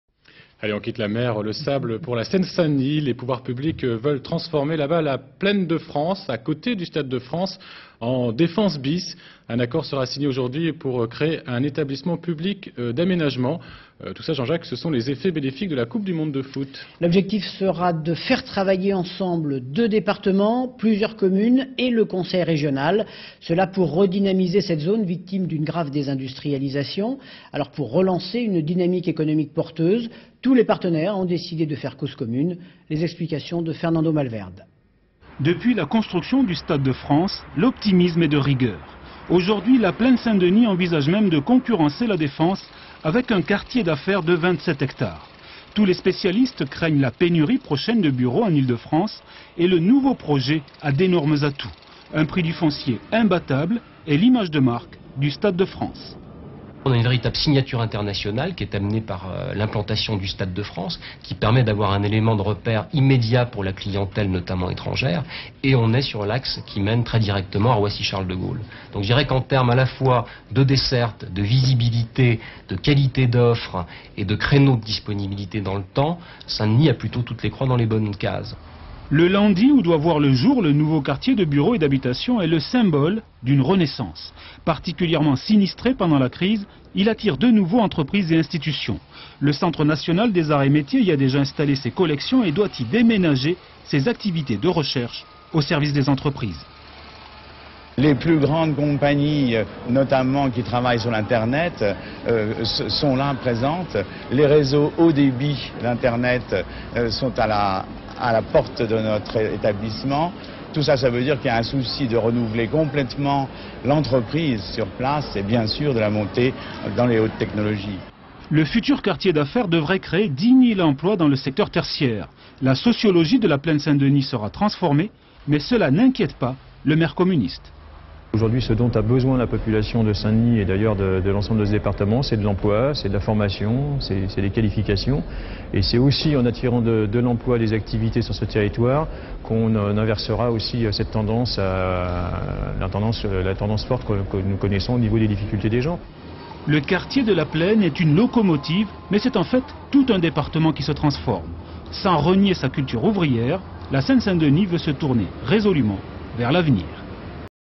Saint Denis : démarrage économique
19 decembre 2000
Les pouvoirs publics projettent de transformer La Plaine Saint Denis, près du Stade de France, en quartier d'affaires comme à La Défense. Dans cette perspective, un accord sera signé pour créer un établissement public d'aménagement avec pour objectif de faire travailler ensemble deux départements, plusieurs communes et le Conseil régional, afin de redynamiser cette zone victime d'une grave désindustrialisation. Pour relancer une dynamique économique porteuse, tous les partenaires ont décidé de faire cause commune. Explications.
Depuis la construction du Stade de France, l'optimisme est de rigueur : La Plaine Saint Denis envisage même de concurrencer La Défense avec un quartier d'affaires de 27 ha. Tous les spécialistes craignent la pénurie prochaine de bureaux en Ile de France et le nouveau projet a d'énormes atouts : un prix du foncier imbattable et l'image de marque du Stade de France.
Interview de Thierry LAROUE PONT, expert immobilier d'entreprise JONES LANG la Salle, sur la clientèle étrangère, attirée par le Stade et la proximité de Roissy.
Le LANDY, où doit voir le jour le nouveau quartier de bureaux et d'habitations est le symbole d'une renaissance. Le Centre National des Arts et Métiers doit y déménager ses activités de recherche au service des entreprises. Interview de Jean BEDIN, directeur de recherche et innovation CNAM sur la présence des entreprises.
10 000 emplois devraient être créés dans le tertiaire. La sociologie de La Plaine Saint Denis sera transformée, mais cela n'inquiète pas le maire communiste.
Interview de Patrick BRAOUEZEC sur les besoins de la population en emploi et formation.
Sans renier sa culture ouvrière, la Seine Saint Denis veut se tourner résolument vers l'avenir. Images d'archive INA